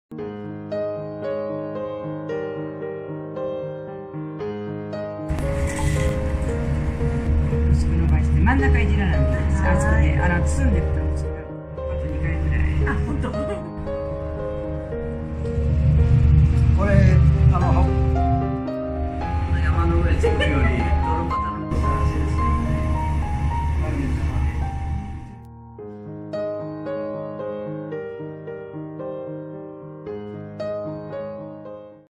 【しあわせ信州SNS動画】しあわせ信州SNS発信講座受講生作品
【制作者 武田さんのコメント】
みぞれ混じりの上越からはちょっと遠かったけど、焼きたてのおやきはとても美味しかったです。